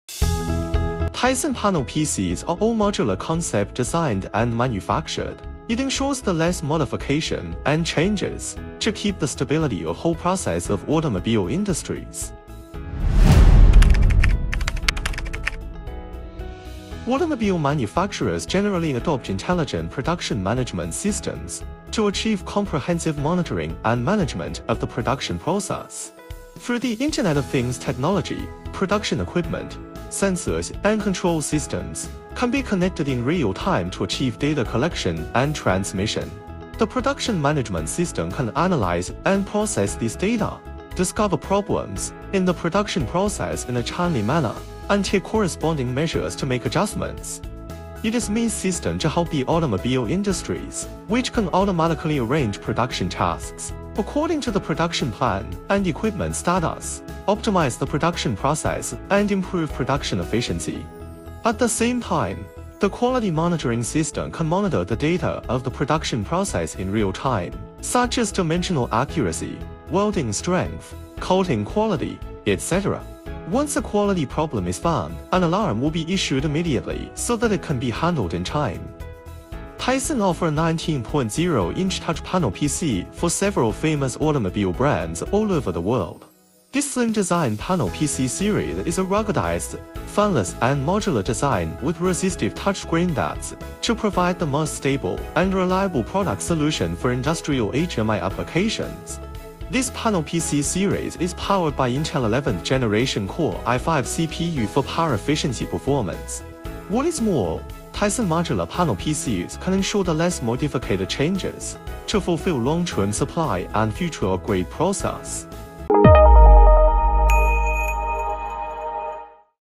Taicenn Panel PC used in smart production line of Automobile industry
automobile manufacturers generally adopt intelligent production management systems to achieve comprehensive monitoring and management of the production process. Through the Internet of Things technology, production equipment, sensors and control systems can be connected in real time to achieve data collection and transmission. The production management system can analyze and process this data, discover problems in the production process in a timely manner, and take corresponding measures to make adjustments.
It is MES system to help the automobile industries, which can automatically arrange production tasks according to the production plan and equipment status, optimize the production process and improve production efficiency. At the same time, the quality monitoring system can monitor the quality data of the production process in real time, such as dimensional accuracy, welding strength, coating quality, etc. Once a quality problem is found, an alarm will be issued immediately so that it can be handled in time.

TAICENN offer a 19.0 inch touch panelpc for several famous automobile brands all over the world. This slim designed Panel PC series is a ruggedized, fanless & modular design with resistive touchscreen that’s to provide the most stable and reliable product solution for industrial hmi applications. This touch Panel PC is powered by Intel’s 11th Gen. Intel Tiger Lake UP3 SoC Core i5 processor for power-efficiency performance.

What’s more, taicenn Modular designed Panel PCs can ensure the less modification changes to fulfill long-term supply and future upgrade process, which is very important to keep the stability of the whole production line of automobile industries.